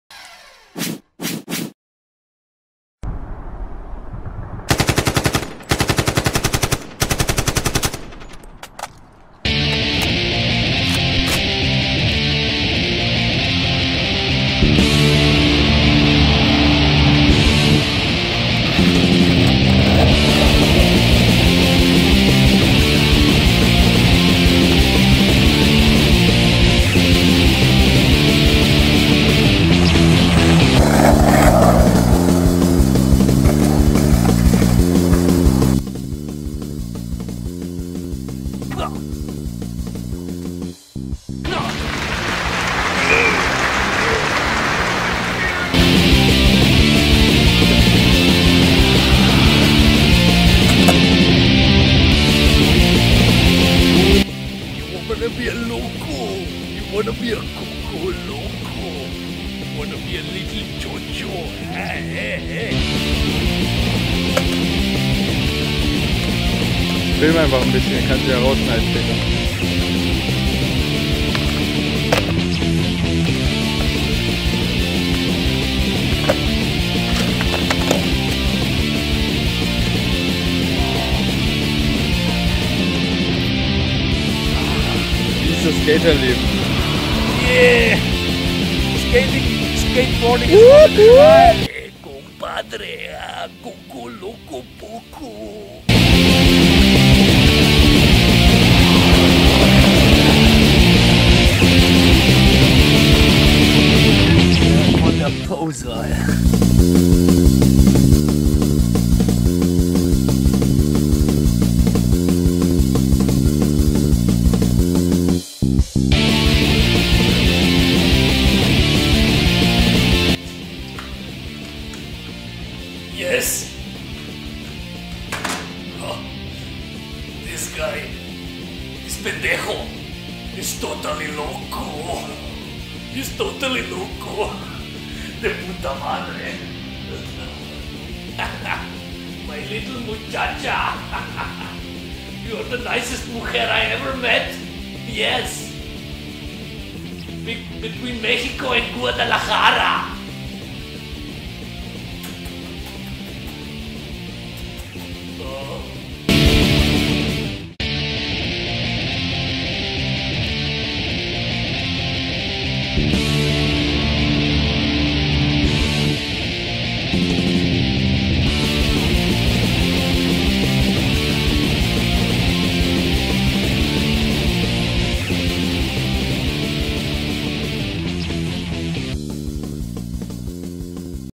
SKATEBOARD POSING TUTORIAL for beginners by Ernesto Romél first time at the board.
Posing tutorial for beginners by Ernesto Romél ! (YEFMEX -yet another filmmaking experiment )